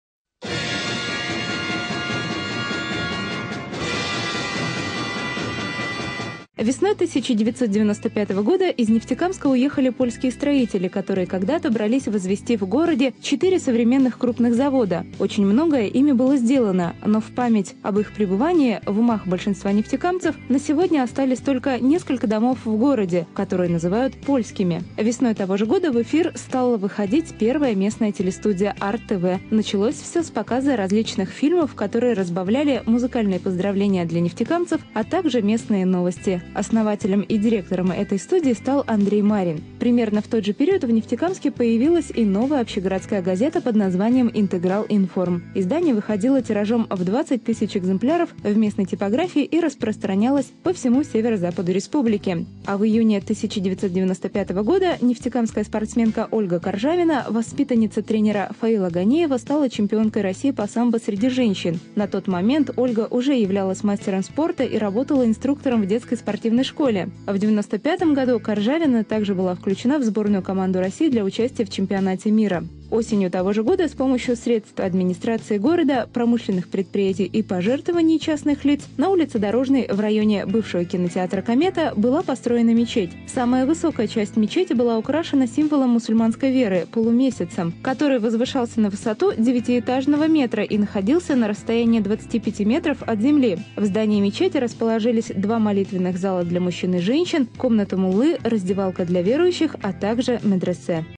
ХРОНИКИ ГОРОДА. ВЫПУСК №36
Телеканал NEXT-TV продолжает выпуск исторических хроник, приуроченных к юбилею города. Сегодня в эфире тридцать шестая  историческая справка, в которой рассказывается о том, как в городе появились польские дома, мечеть и еще одна городская газета.